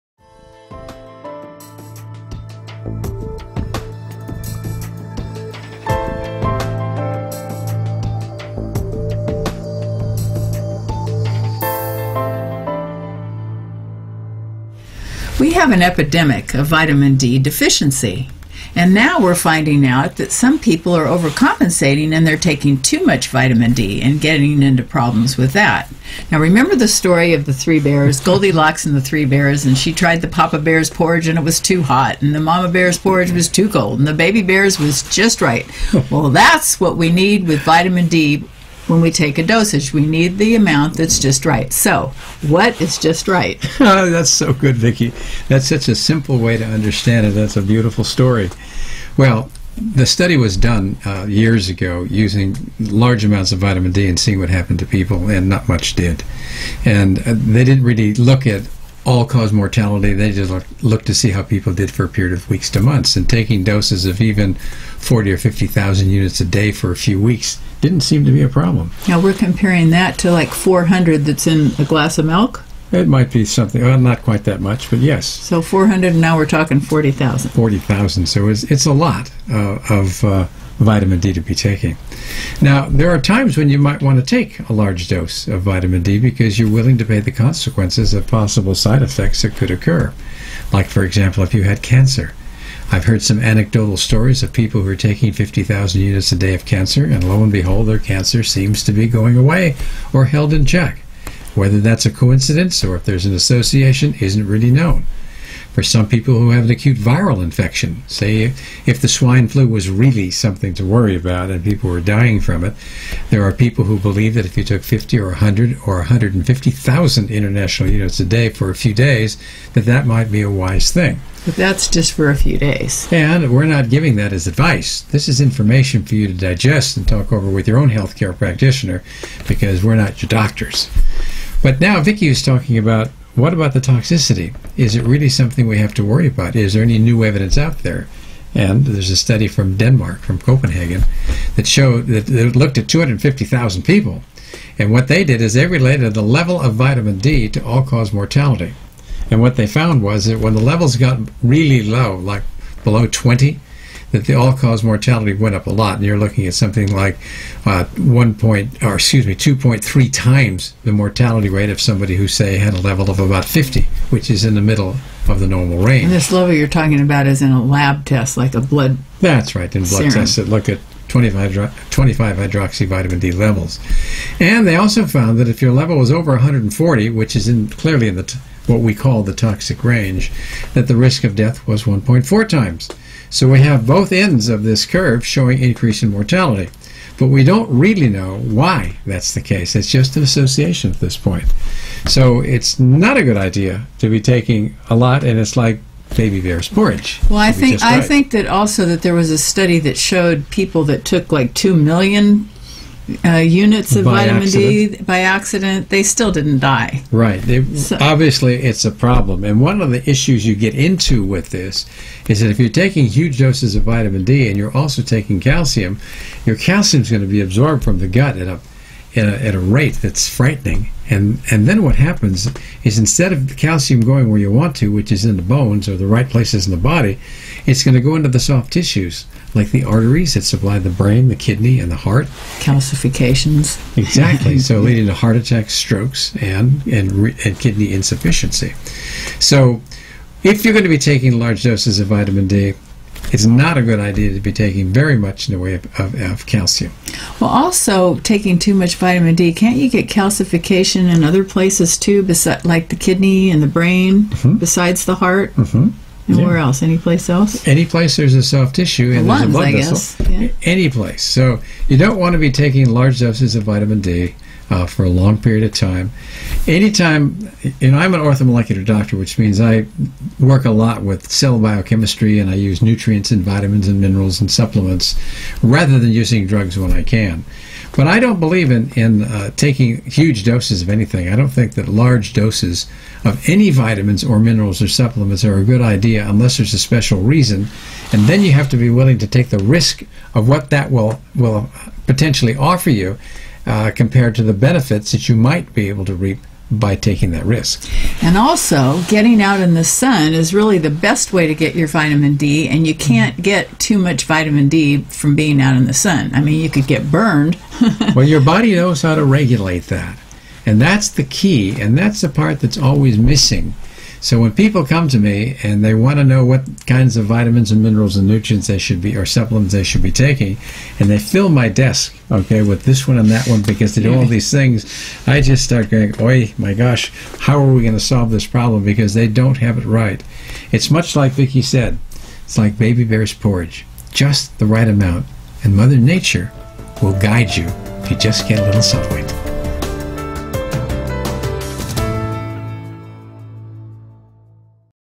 How much Vitaman D is too much?
We know that too little vitamin D is not healthy, but how about too much? Until recently, it has been believed that megadoses of vitamin D might not be a good idea, but that it was relatively harmless. However, a study in the Journal of Clinical Endocrinology & Metabolism looked at 250,000 people from Denmark showing that levels below 20 nmol are associated with a 2.3 times increased risk for dying and that levels above 140 also had an increased mortality of 1.4 times above the ideal level of 50.

Unless there is an advantage of taking megadoses of vitamin D for some special reason, it is wise to keep replacement dosages to what is needed to maintain a level of 50 nmol. Some examples where especially high dose vitamin D3 supplementation might be considered include viral infections, cancer, and autoimmune disorders.